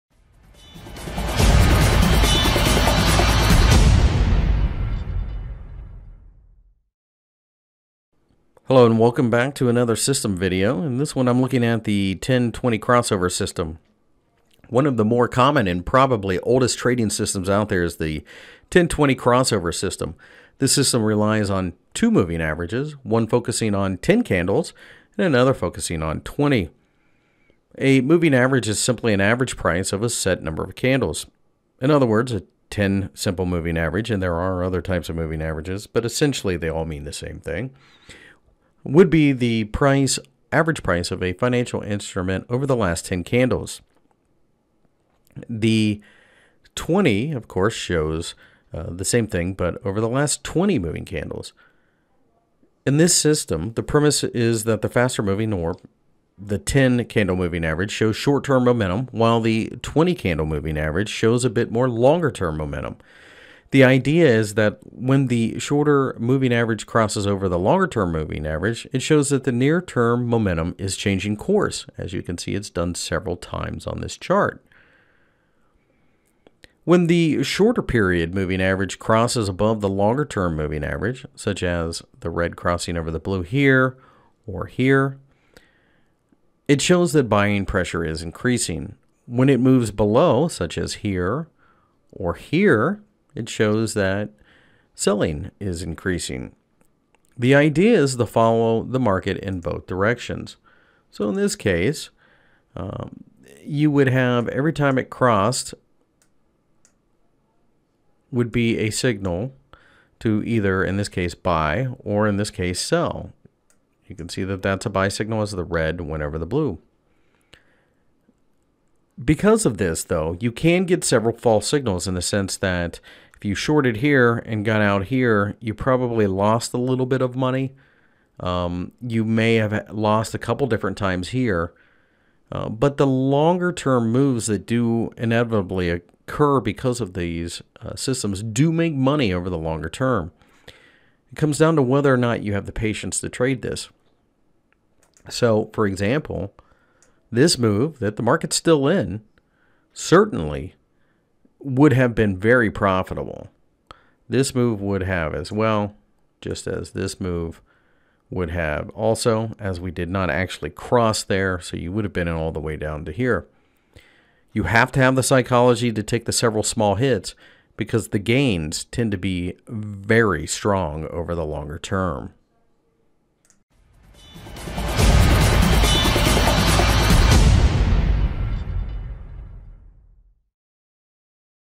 How to trade moving averages crossover - 10/20 Crossover Trading System
Best + Low Spread Broker (instant withdraw)
Ultimate moving average crossover strategy

10/20 Crossover Trading System is probably the most common and oldest trading system which relies on two moving averages: one focusing on 10 candles and the other one focusing on 20. 10 and 20 candle moving average shows short-term momentum and long-term momentum respectively. This trading system sometimes gives false signals. You need to have the patience to make money following this system because in the long term this system tends to work.

In this video, you get to know how the 10/20 crossover trading system works, how to utilize this system to make money and some other basics.